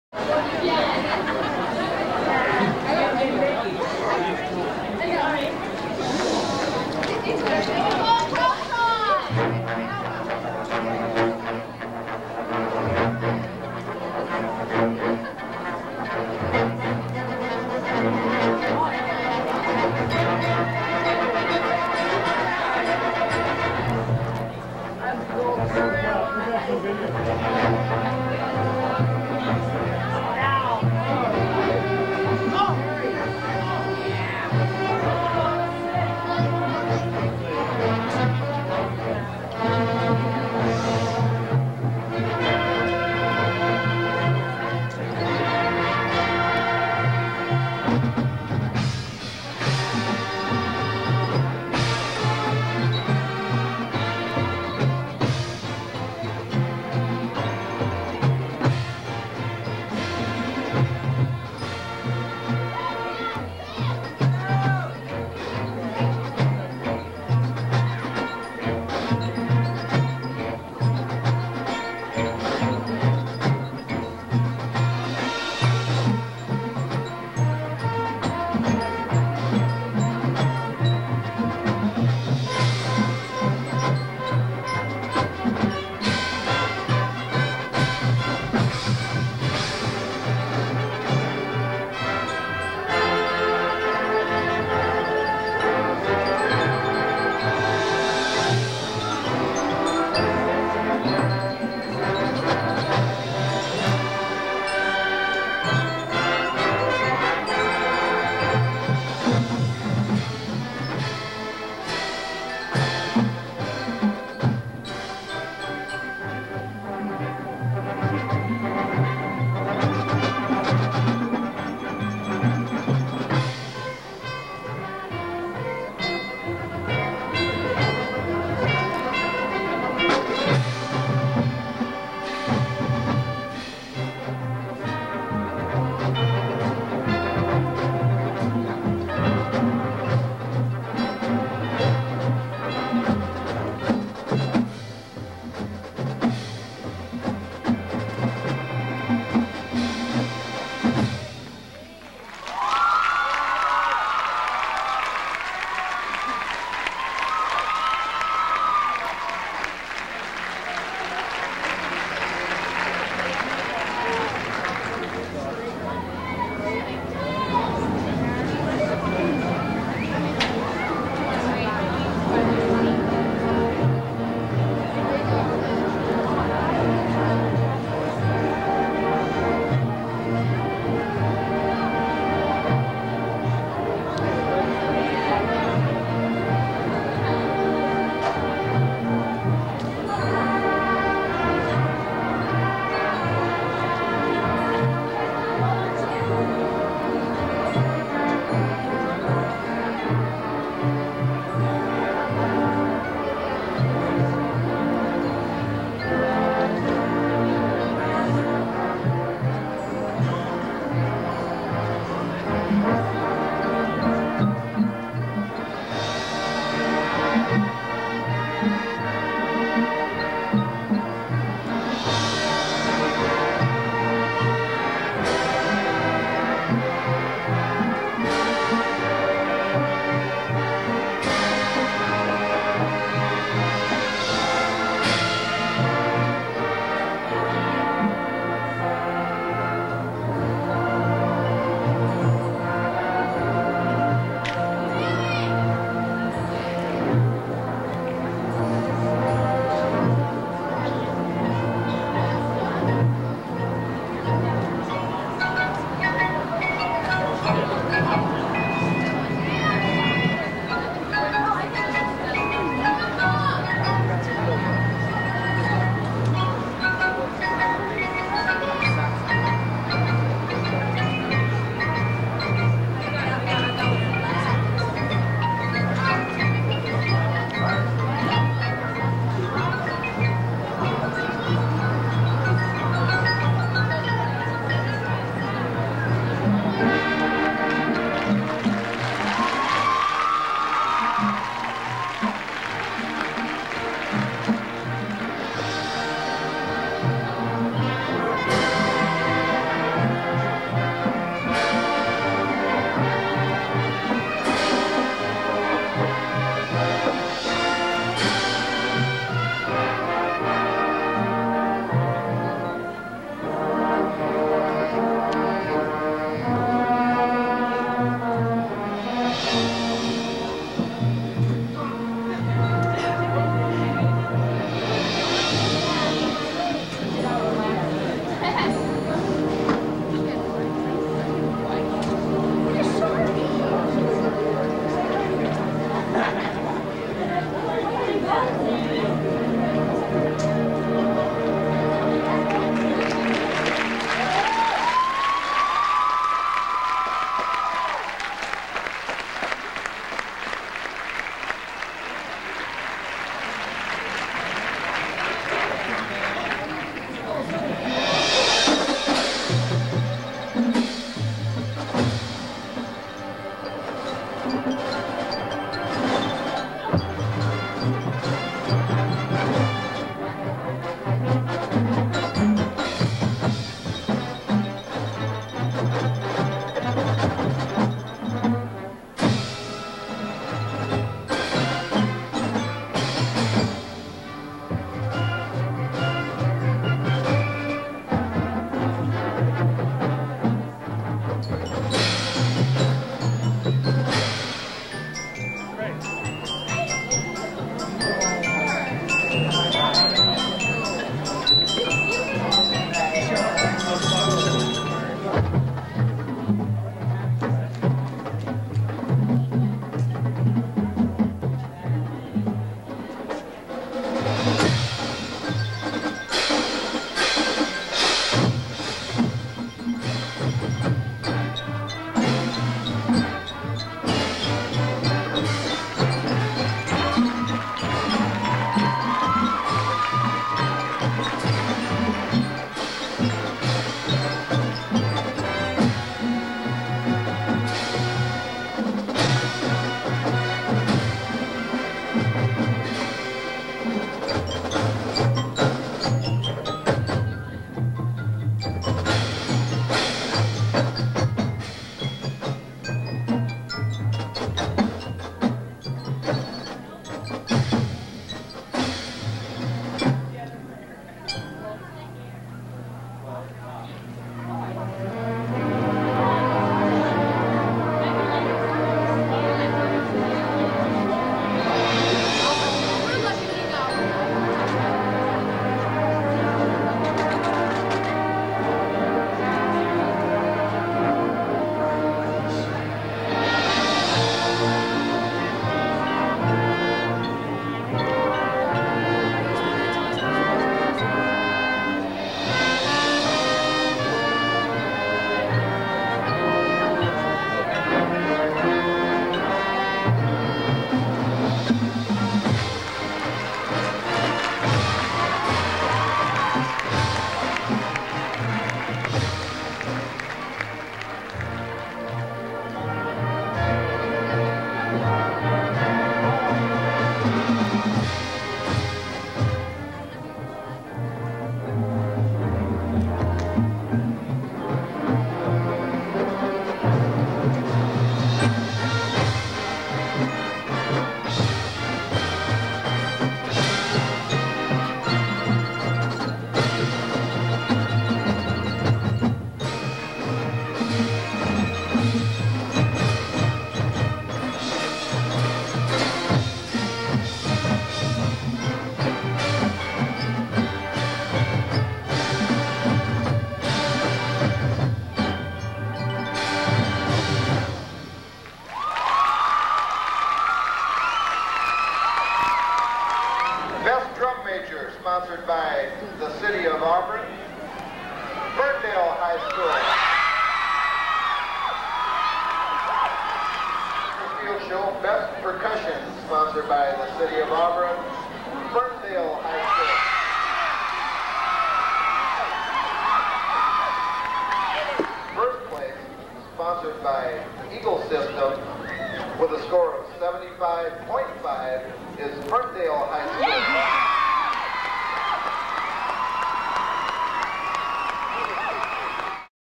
1991 Ferndale High School Marching Band
1st Place AA, Best Drum Major, Best Percussion